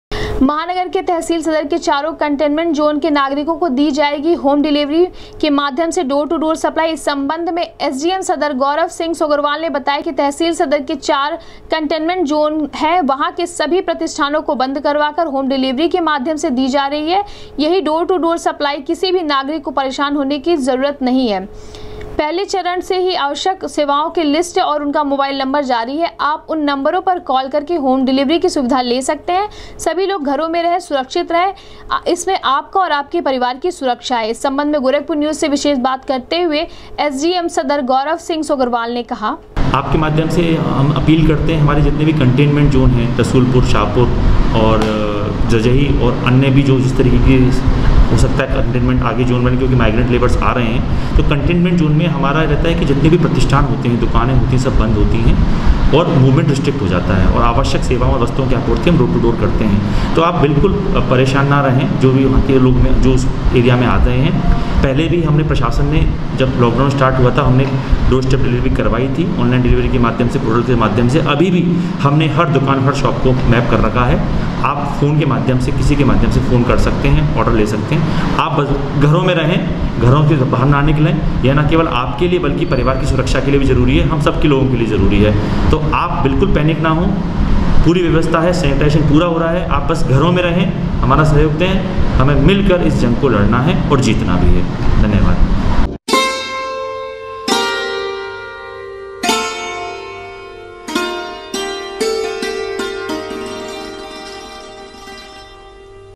महानगर के नागरिको से एसडीएम सदर ने कहा,आवश्यक सेवाओं की लिस्ट और उनका मोबाइल नंबर है
महानगर के नागरिको से एसडीएम सदर ने कहा,आवश्यक सेवाओं की लिस्ट और उनका मोबाइल नंबर है जारी, उन नम्बरों पर कॉल करके ले सकते है होम डिलीवरी की सुविधा  GORAKHPUR NEWS  -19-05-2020